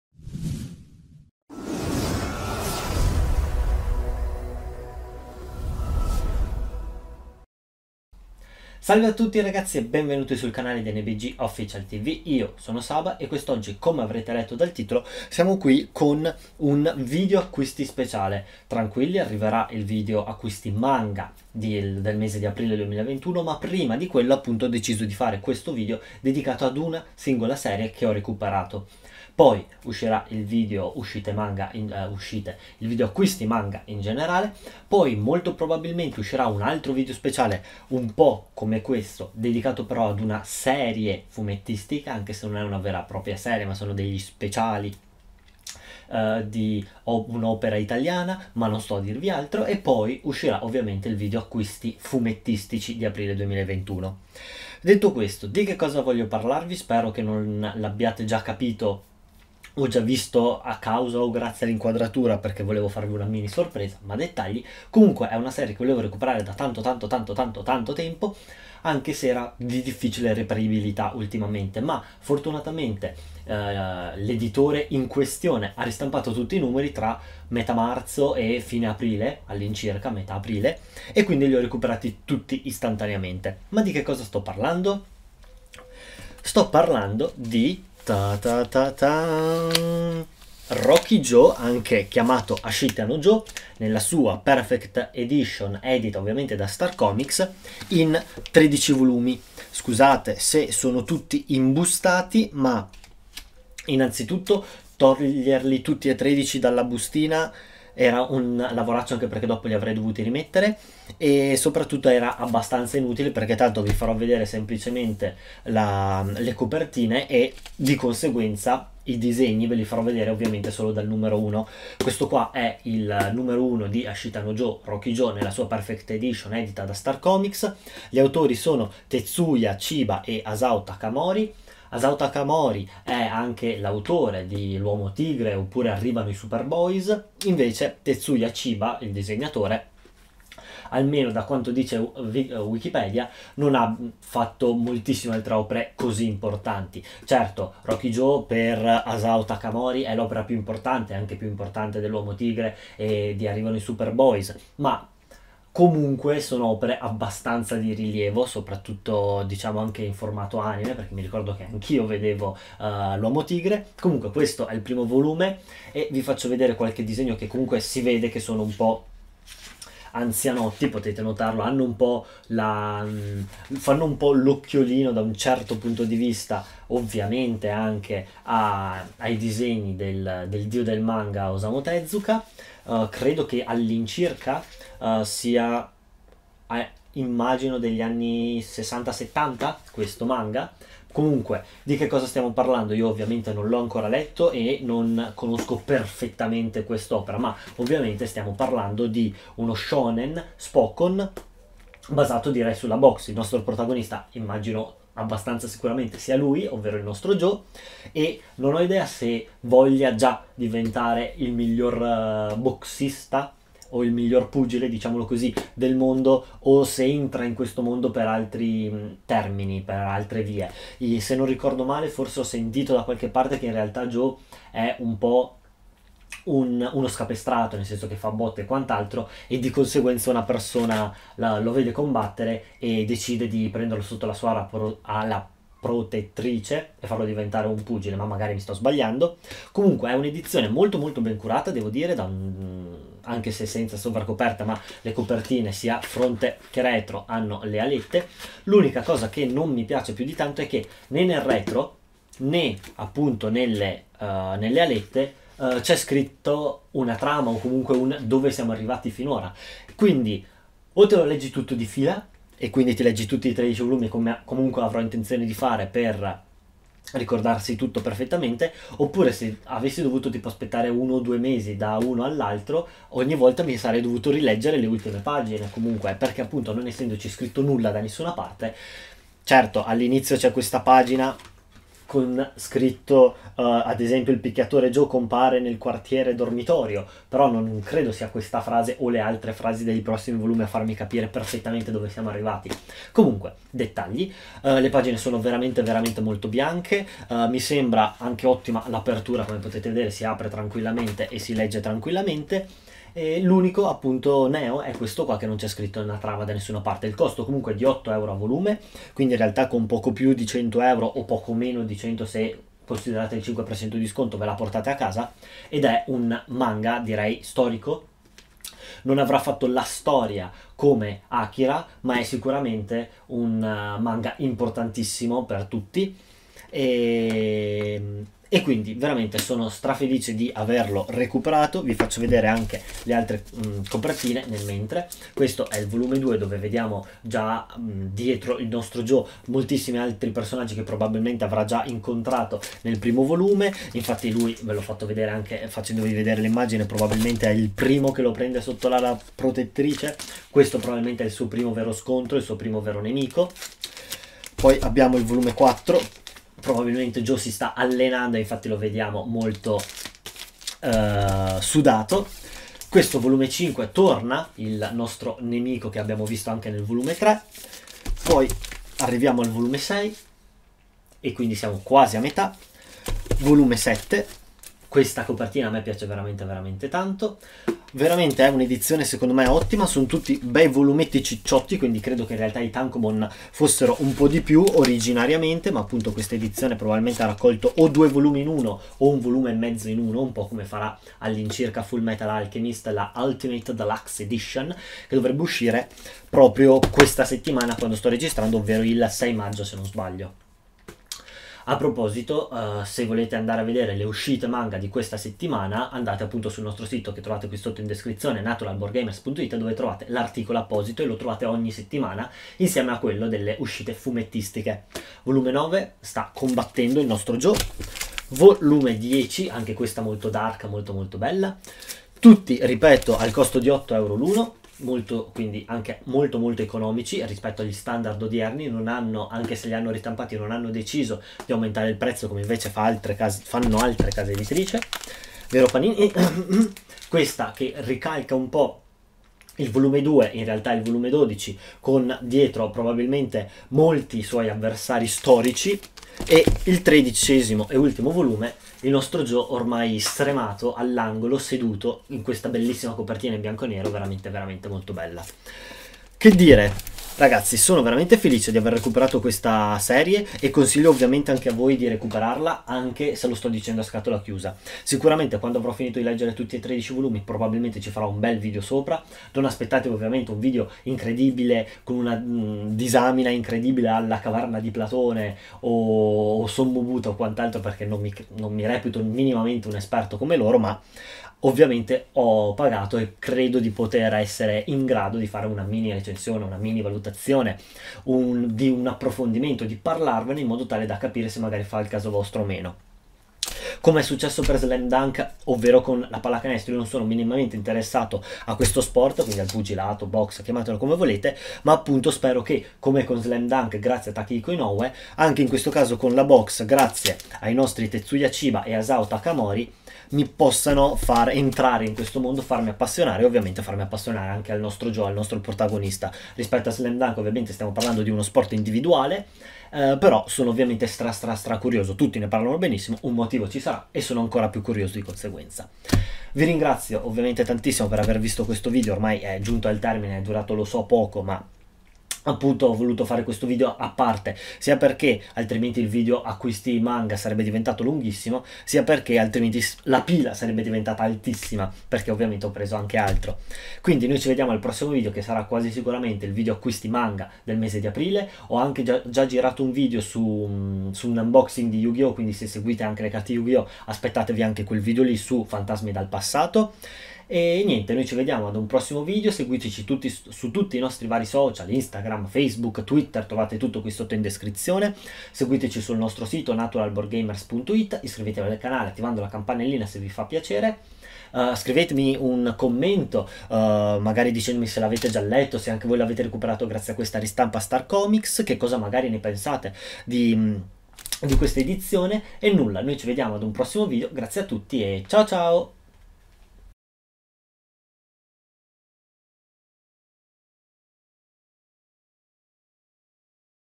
Acquisti Manga - Recupero di un Grande Classico
Eccoci con un nuovo Video Acquisti sui Manga. In questo caso si tratta di un recupero di uno dei Grandi Classici scritto da Asao Takamori e disegnato da Tetsuya Chiba. Il nosto Saba ci delizierà con questa edizione davvero speciale!